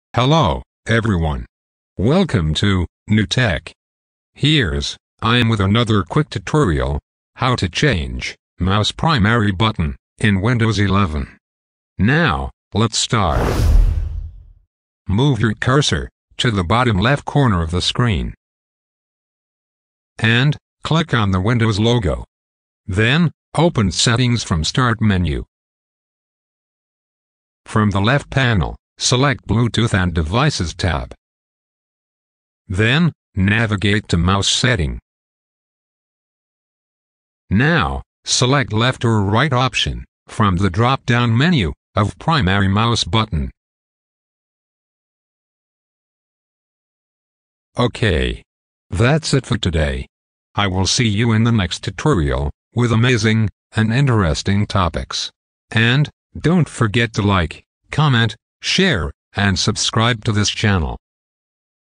How to change mouse primary button in Windows 11 | Swap Left and Right Mouse Buttons
In this video you will learn about how to change or switch the mouse primary buttons in any Windows 11 computer. When your left or right button of mouse doesn't work then swapping the action of mouse is better idea to run the mouse.